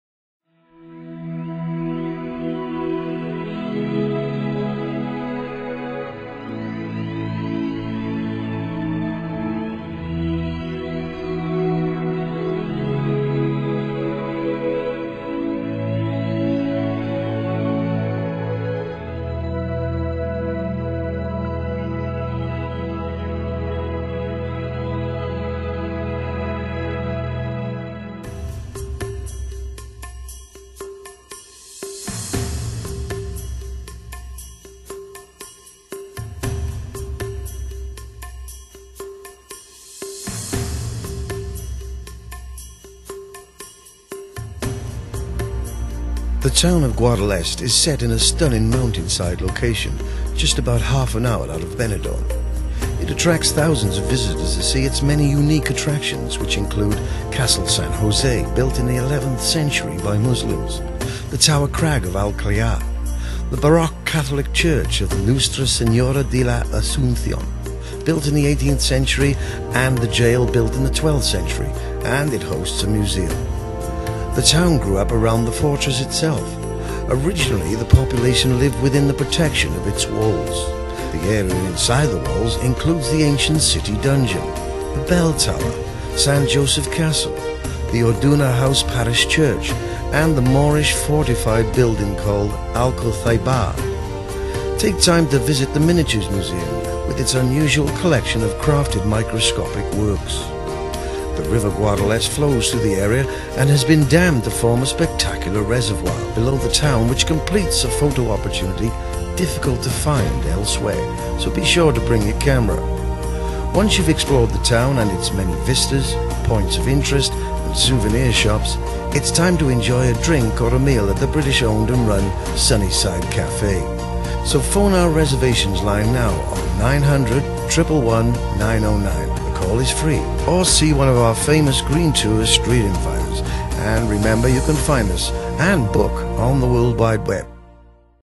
GUADALEST
THIS AMAZING MOUNTAINTOP VILLAGE IS AN ABSOLUTE MUST TO VISIT WITH AN IMPRESSIVE HISTORY & CULTURE.